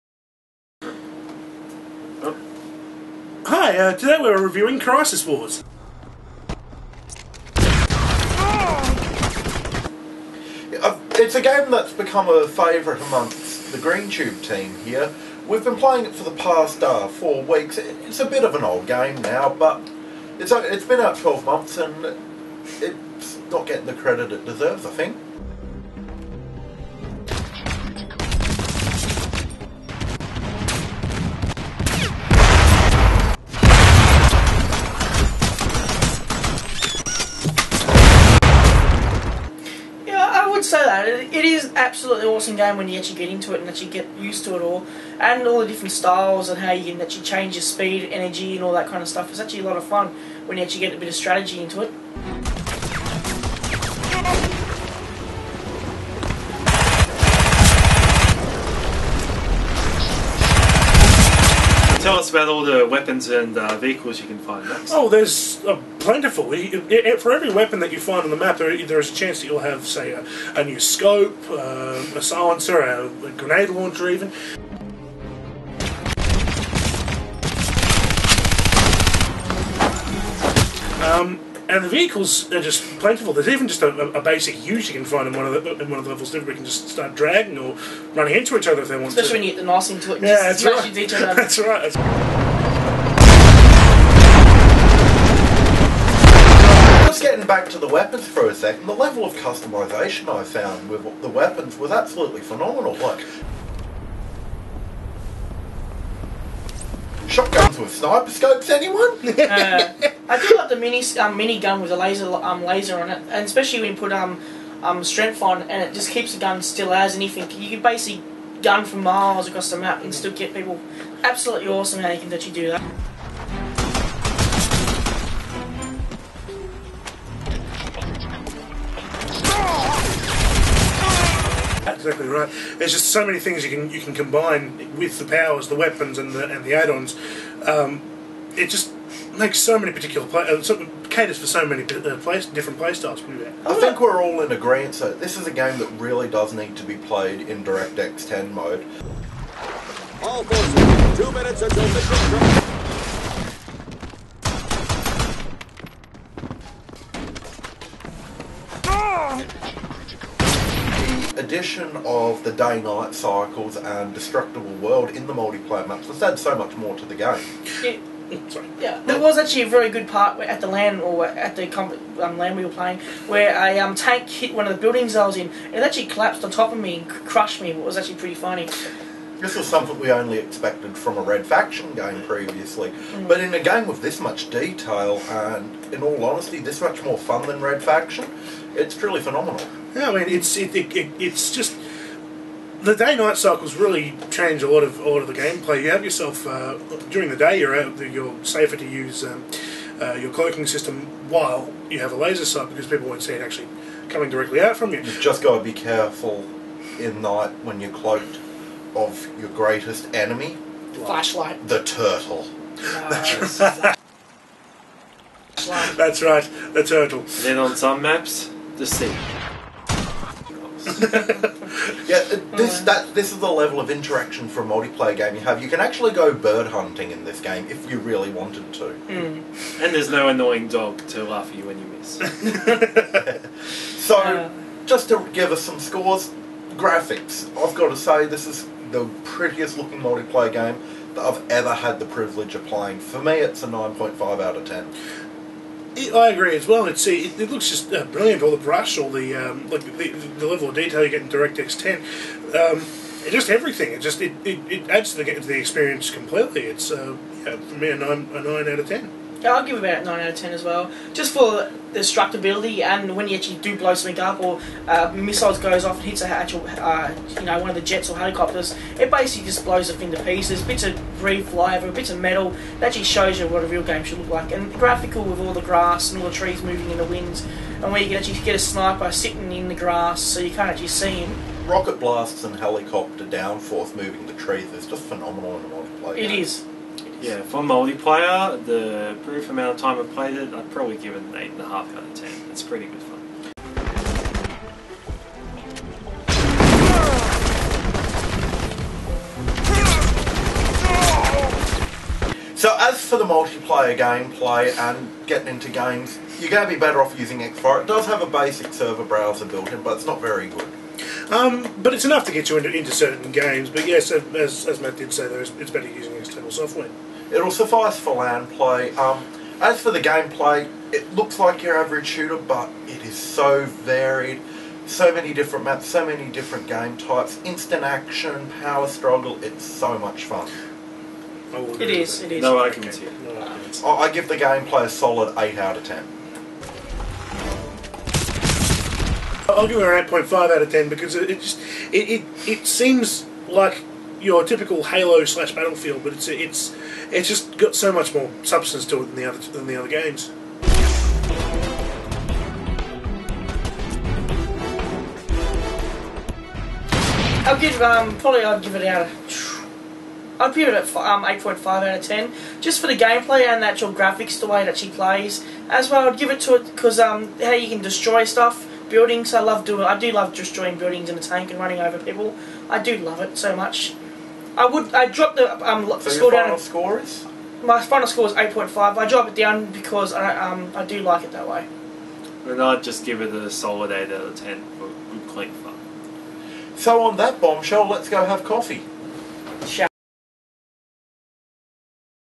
Crysis Wars Review GreenTubeTV
This is the GreenTubeTV video review of Crysis Wars.

We love this game so much, that the entire team wanted to get in on the action :)